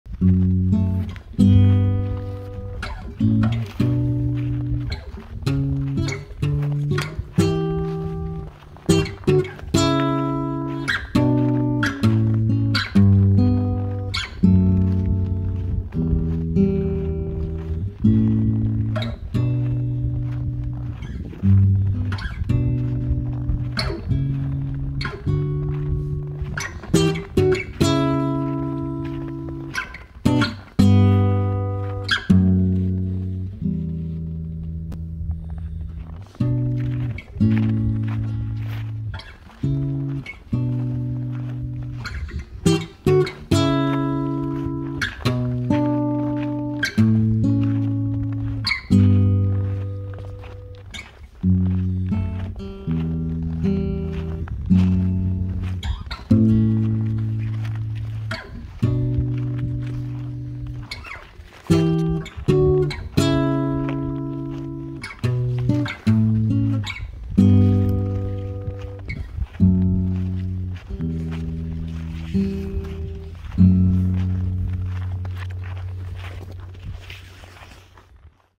Sunny winter day in a bog
Spending time in nature is the best way to relax and recharge. Estonia has a lot of beautiful bogs. This is a magical experience!